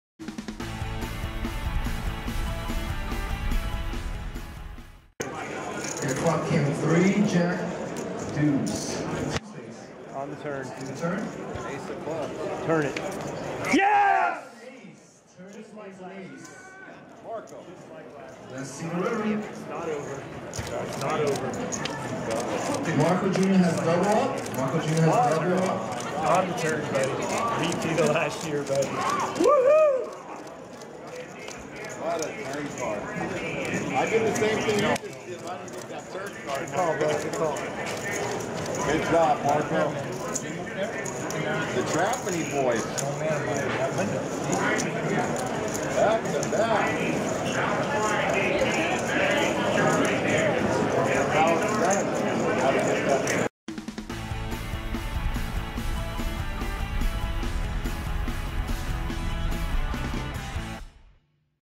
Marko Trapani Jr. with an Emotional Double Up on Day 2
Marko Trapani Jr. doubles up late on Day 2 of the event his father made so great, the Marko Trapani Bay 101 Shooting Star.